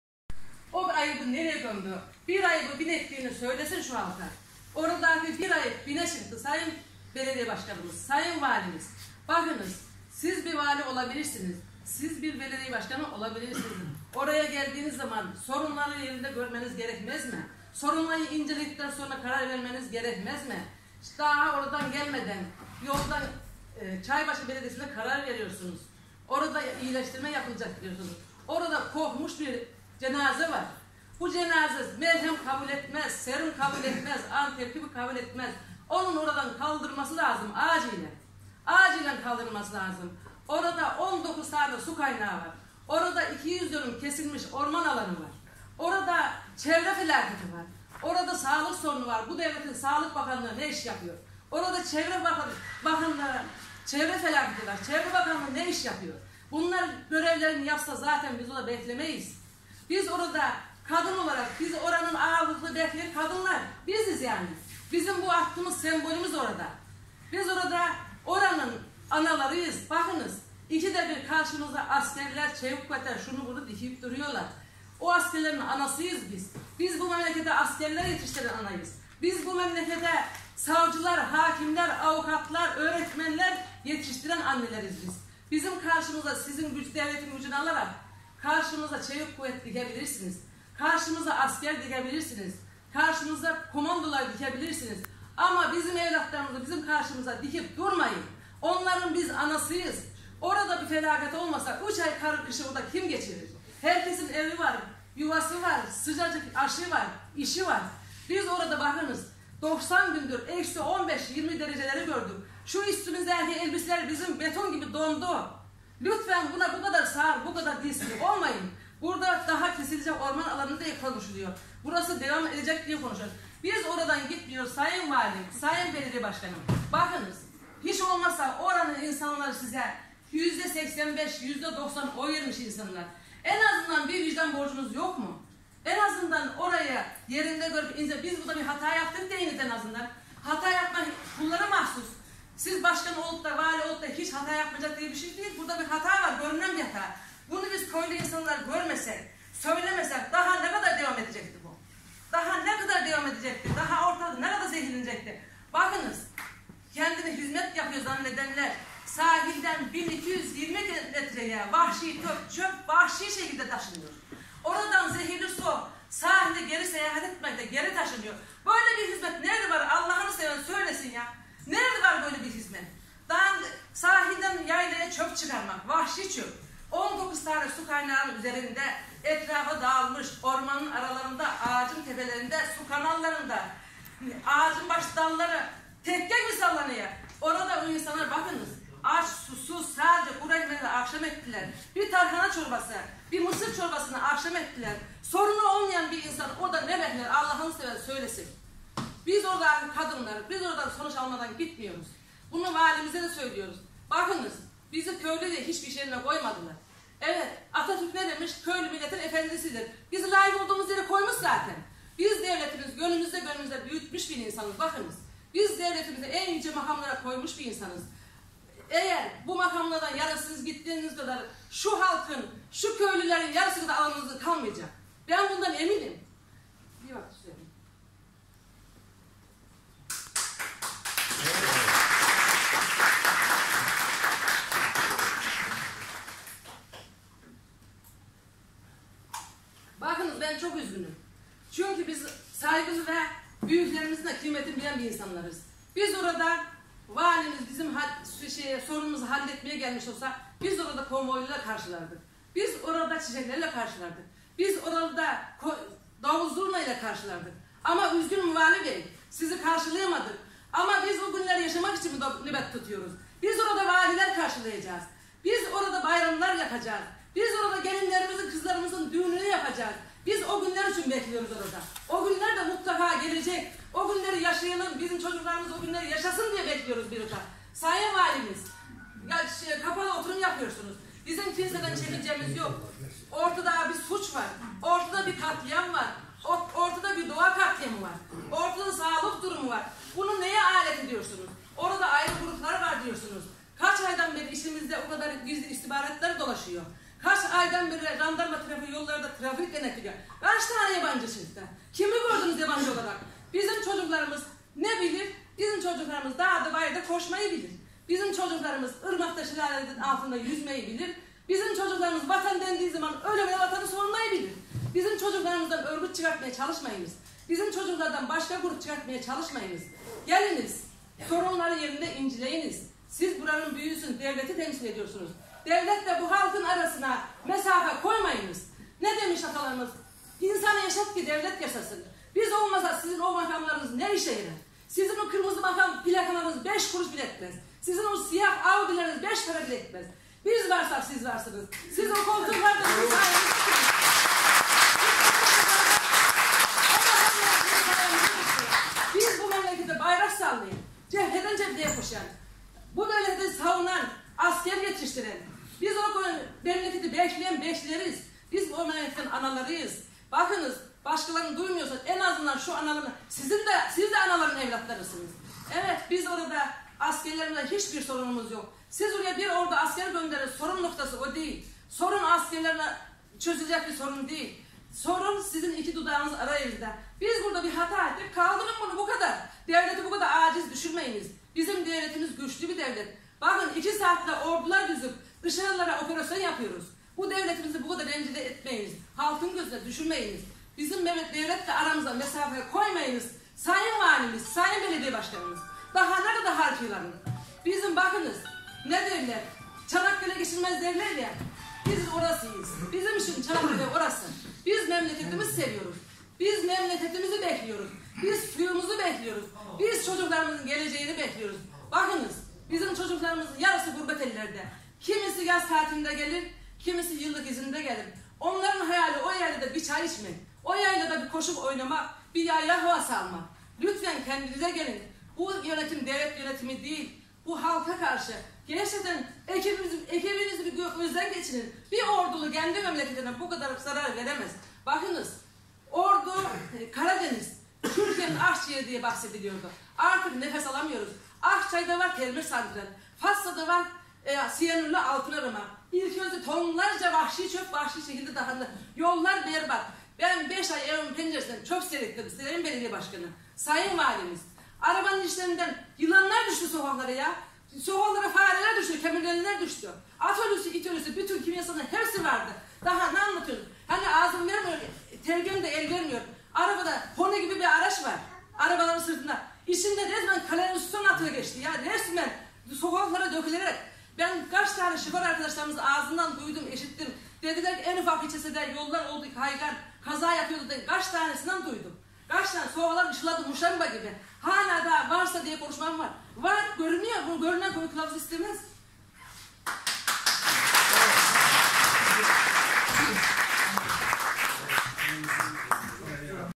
Çöp alanı mağduru Ordu ili Çaybaşı İlçesi İlküvez mahallesi halkının isyankar çığlığı
Ordu'da çöp alanı mağduru halime gelen Çaybaşı İlçesi İlküvez mahallesi halkının isyankar çığlığı Ekoloji Birliği'nin Ordu Fatsa'da yapılan Orta Karadeniz Bölge Toplantısında'da kendini bir kez daha duyurdu.